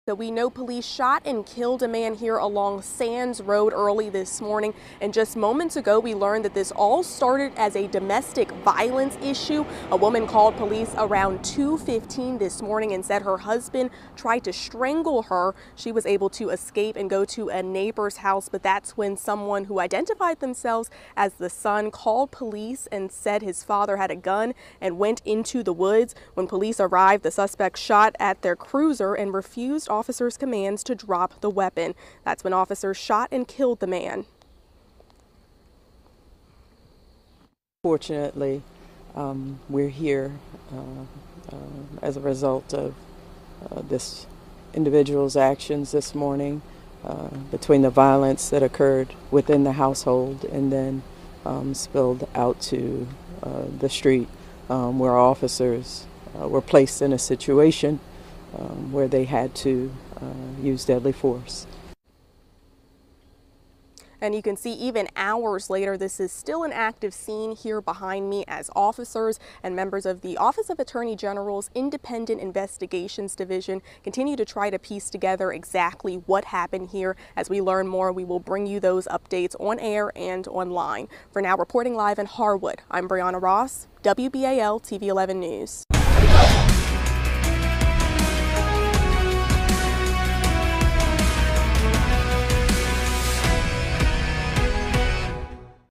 Update: Man fatally shot by police in Harwood
Officers called early Saturday to a domestic dispute fatally shot an armed man, Anne Arundel County police said. County police said officers were called around 2:15 a.m. to the 4100 block of Sands Road in the Harwood area. Police said a woman told them that her husband tried to strangle her. She was able to break free and get to a neighbor's house. Police said officers arrived, heard gunshots and moved to a position of cover when they realized their cars were shot at from behind and beside them. Police said officers repositioned as the suspect emerged from the woods with a gun. Police said officers warned the suspect to drop the gun, but he did not comply and officers shot him.